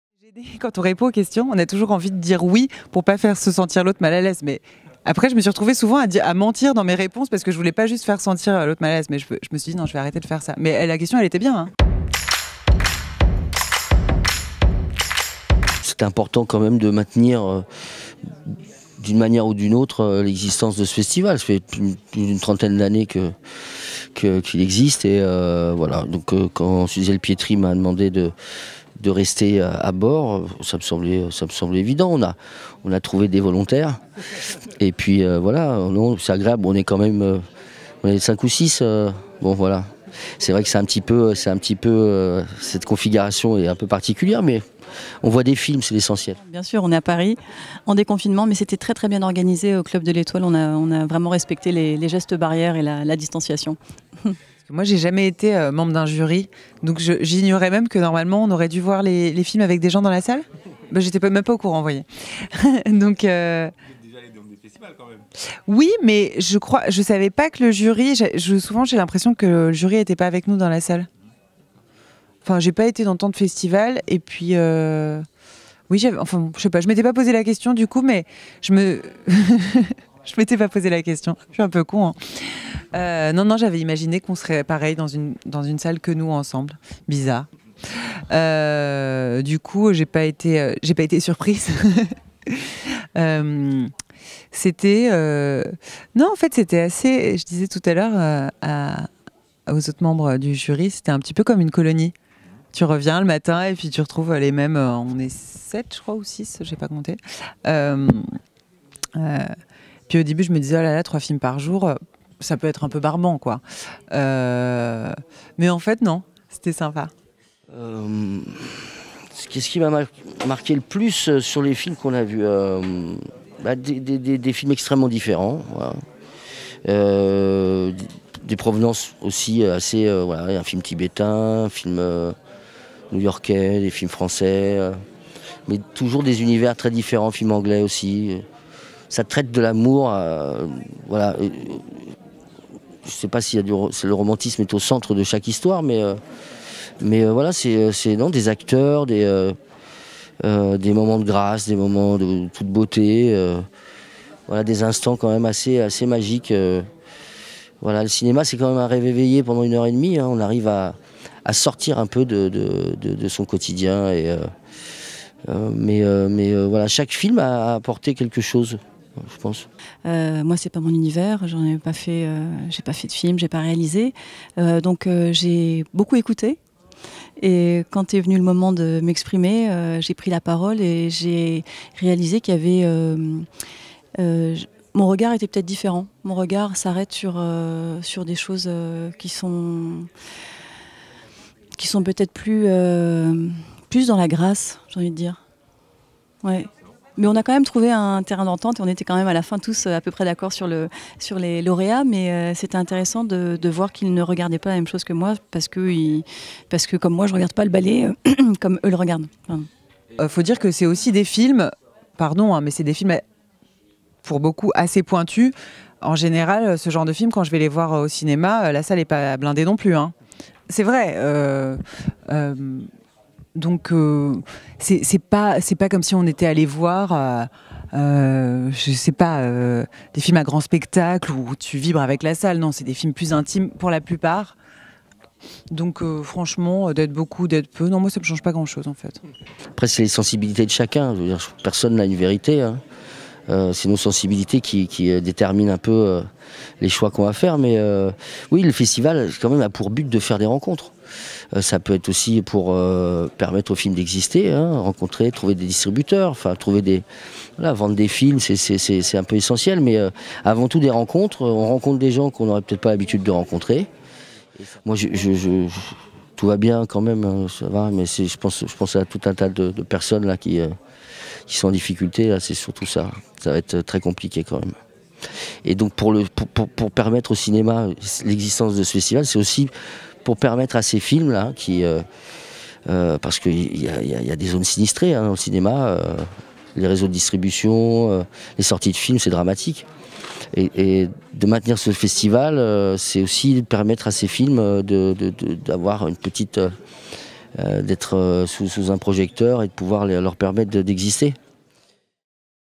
Festival de Cabourg 2020 : Rencontre avec le jury
Nous avons croisé 3 des jury de l'édition 2020 du festival du film romantique que Cabourg, une édition un peu spéciale car hors les murs, à Paris en mode déconfiné...
Benoit Magimel (Président du Jury), Aurélie Dupont (directrice de la danse du ballet de l’Opéra national de Paris) et Doria Tillier nous racontent…